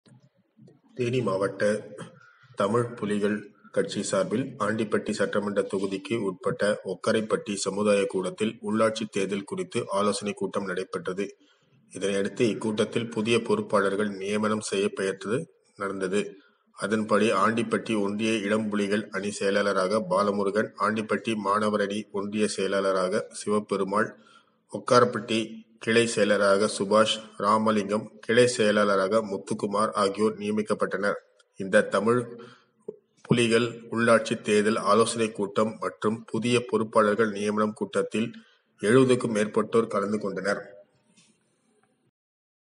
தமிழ் புலிகள் பொறுப்பாளர்கள் கூட்டம்
தேனி கிழக்கு மாவட்ட தமிழ் புலிகள் கட்சியின் சார்பில் ஆண்டிப்பட்டி சட்டமன்ற தொகுதிக்கு உட்பட்ட ஒக்கரைபட்டி சமுதாயக் கூடத்தில் உள்ளாட்சி தேர்தல் குறித்து ஆலோசனைக் கூட்டம் நடைபெற்றது. இதனையடுத்து இக்கூட்டத்தில் புதிய பொறுப்பாளர்கள் நியமனம் நடைபெற்றது அதன்படி ஆண்டிபட்டி ஒன்றிய இளம்புலிகள் அணி செயலாளராக பாலமுருகன், ஆண்டிபட்டி மாணவரணி ஒன்றியச் செயலாளராக சிவபெருமாள், ஒக்கரைபட்டி கிளைச் செயலாளராக சுபாஷ், ராமலிங்கபுரம் கிளைச் செயலாளராக முத்துக்குமார் ஆகியோர் நியமிக்கப்பட்டனர். இந்த தமிழ் குளியல் உள்ளாட்சித் தேர்தல் ஆலோசனைக் கூட்டம் மற்றும் புதிய பொறுப்பாளர்கள் நியமனம் கூட்டத்தில் 70-க்கும் மேற்பட்டோர் கலந்துகொண்டு சிறப்பித்தனர்